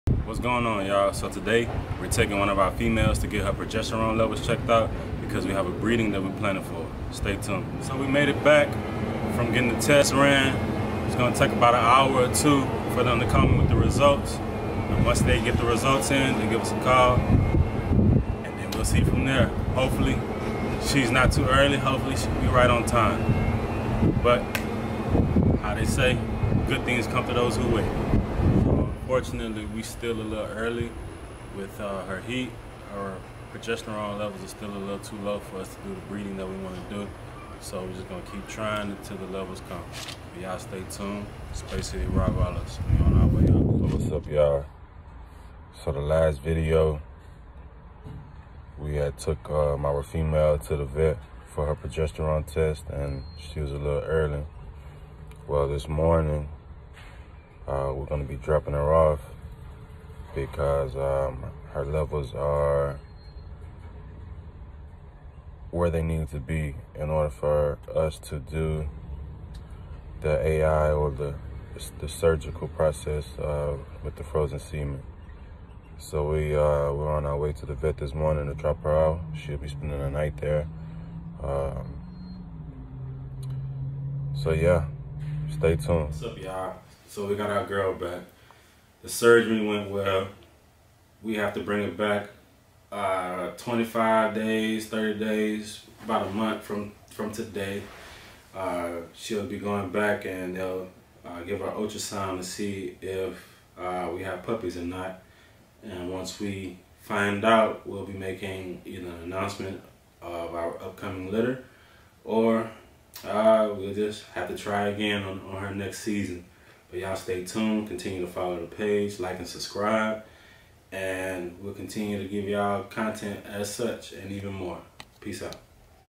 Preparation for our summer 2020 litter
A few weeks back and forth to the vet in preparation for an artificial insemination for our up coming litter.